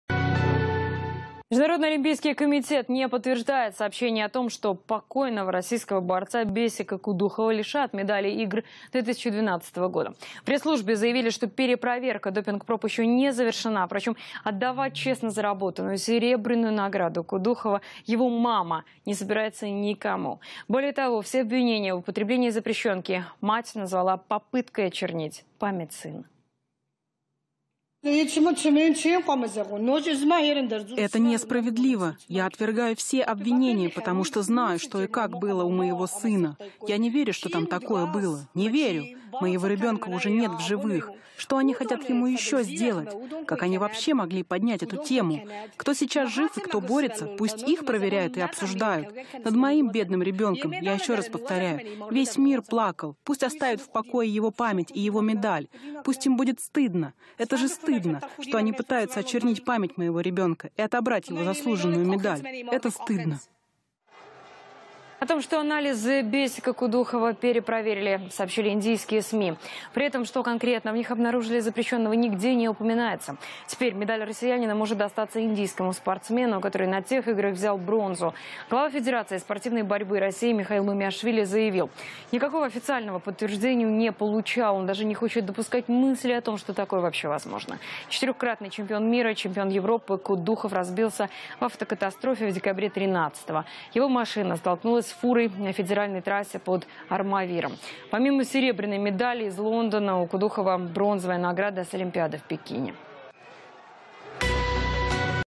МОК не подтвердил лишения борца Бесика Кудухова медали Игр 2012 из за допинга
МОК: Вопрос с медалью погибшего призера решат после перепроверки допинг проб